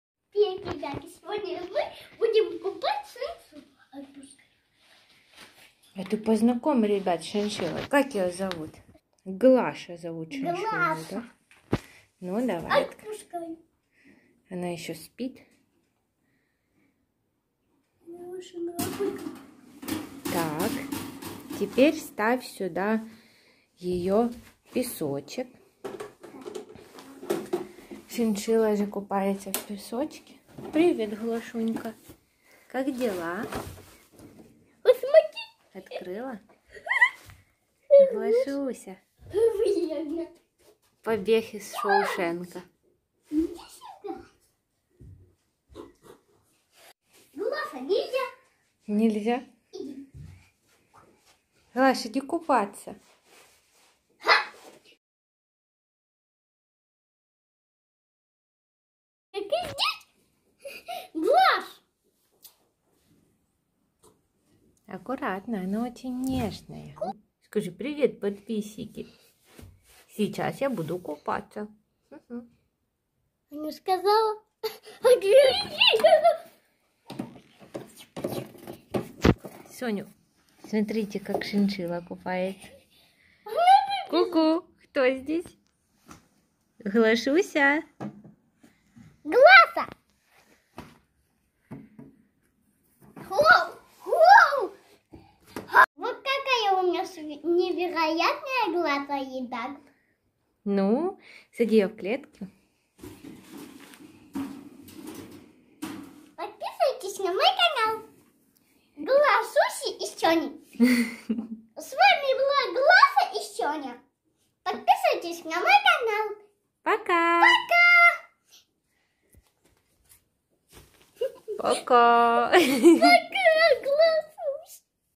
ШИНШИЛЛА - любимый зверек всех детей. Соня играет с Глашей
Знакомьтесь это шиншилла Глаша.
Она очень добрая и забавная.
Любит играть в прятки, бегать по квартире, кушать вкуснятинку, например семечки или изюм 🧡
Шерстку она предпочитает чистить в песке. В видео вы уведите этот процесс. Всё происходит в закрытой емкости. Это связано с тем, что, «плескаясь» в мелком песке с тальком, Глаша поднимает много пыли.
Мы её очень любим и решили поделится с Вами моментами радости, которые приносит эта маленькая шиншилла 🥰